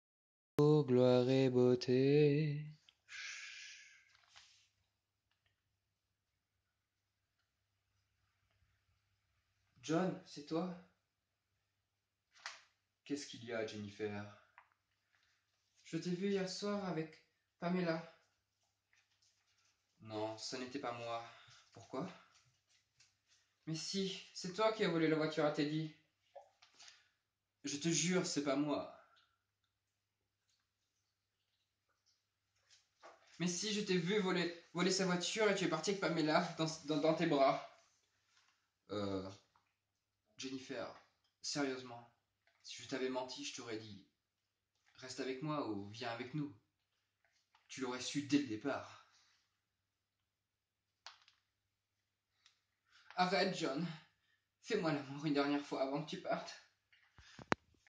Amour Gloire et Beauté
Une parodie des types amour gloire et beauté